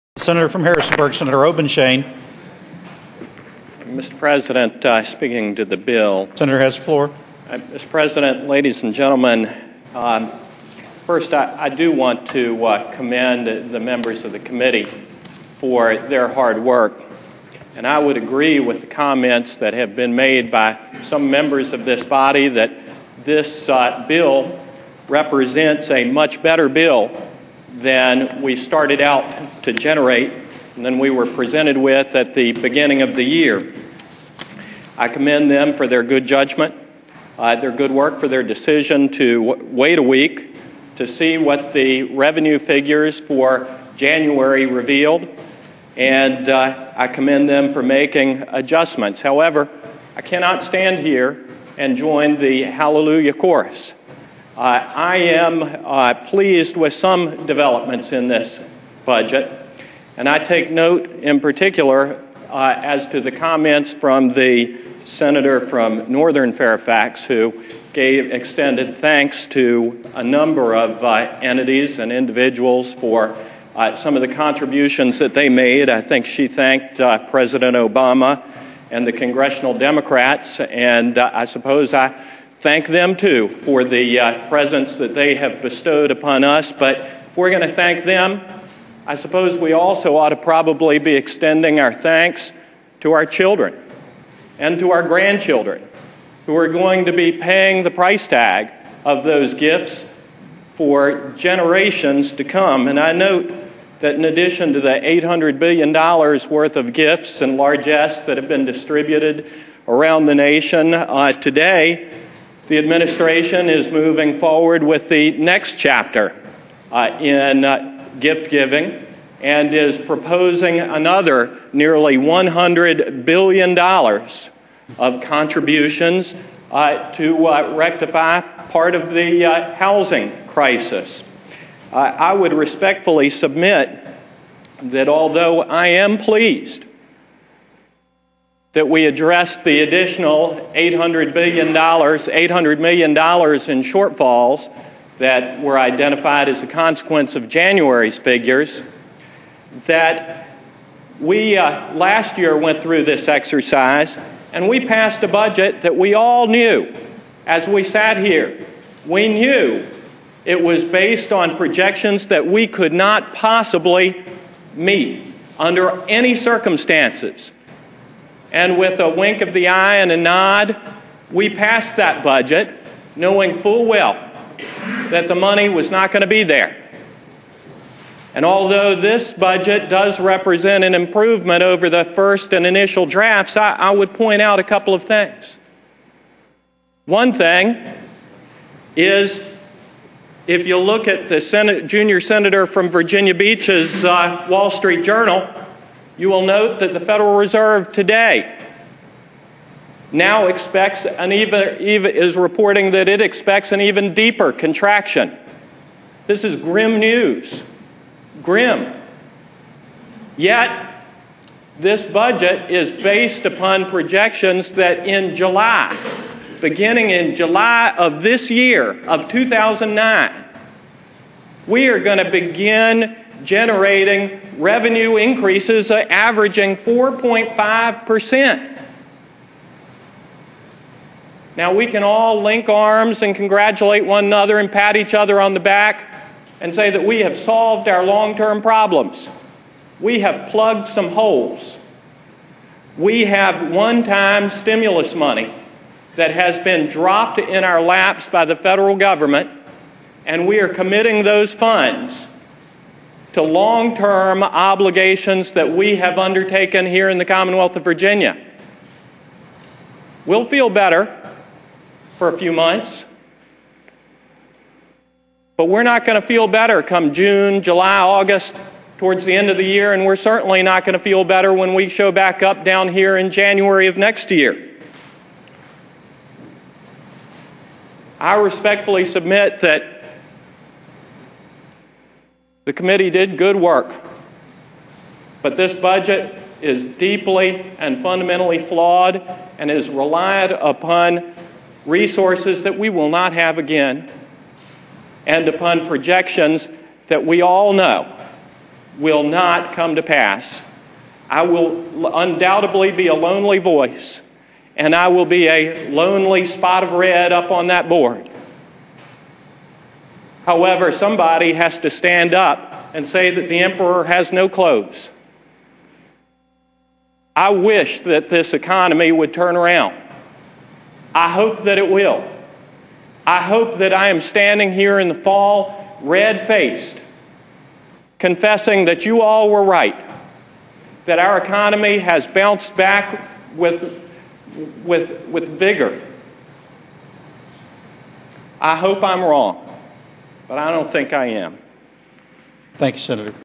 Sen. Mark Obenshain on the Senate Budget
Sen. Obenshain outlines his opposition to the Senate Budget Bill, pointing out the folly of leaning on the federal stimulus package in lieu of undertaking serious structural reforms to the state budget.